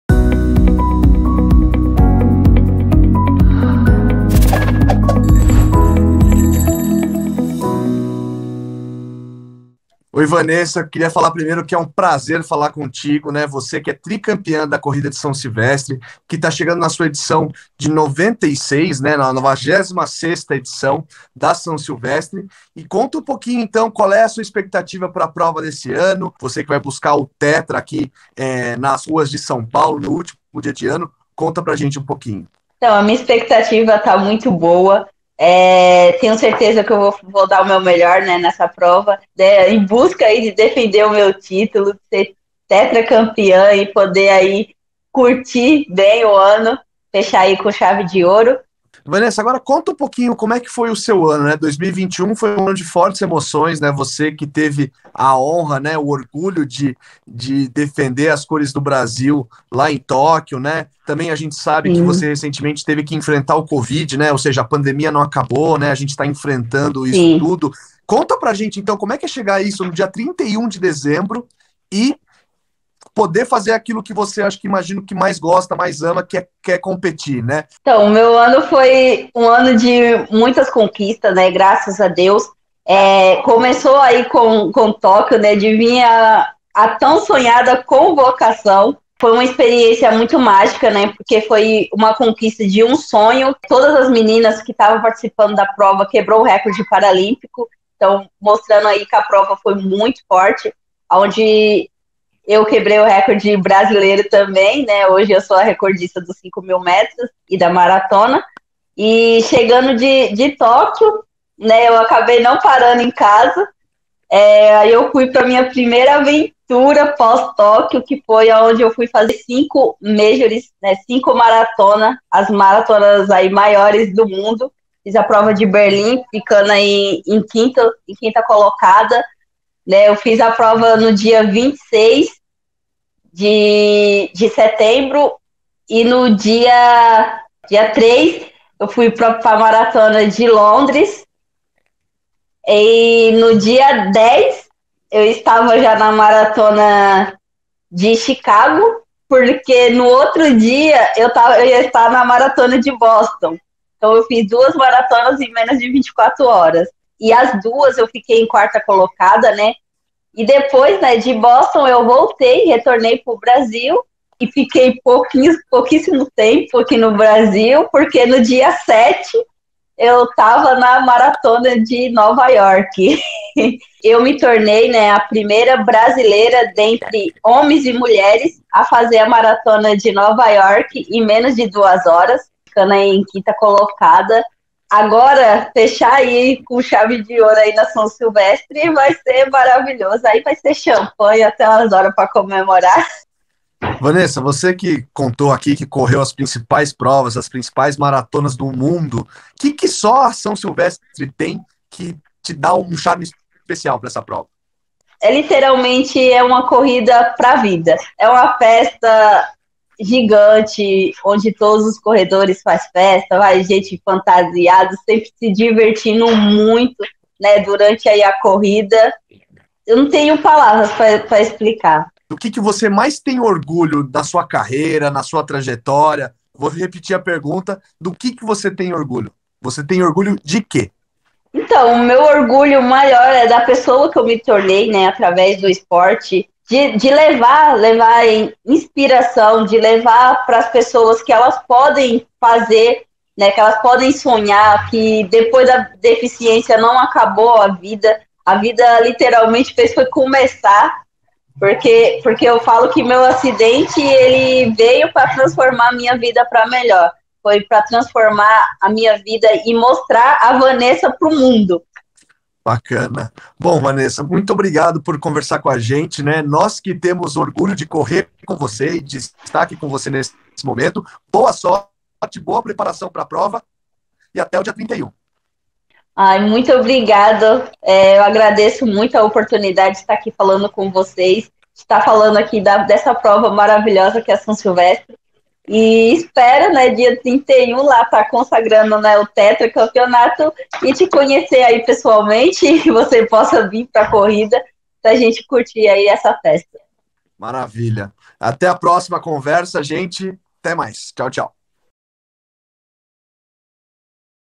Assaí | São Silvestre: Vanessa Cristina Corte
Está chegando a corrida mais tradicional da América Latina! ♿🏁

A competição que é a cara de São Paulo vai fechar o ano de 2021 com sua 96ª edição. E ninguém melhor que a Vanessa, tricampeã da prova e paratleta olímpica, para contar como é essa experiência que tem na diversão dos participantes seu principal diferencial.

Conheça mais sobre a Vanessa e a corrida São Silvestre

Assaí na São Silvestre, orgulho de correr com você!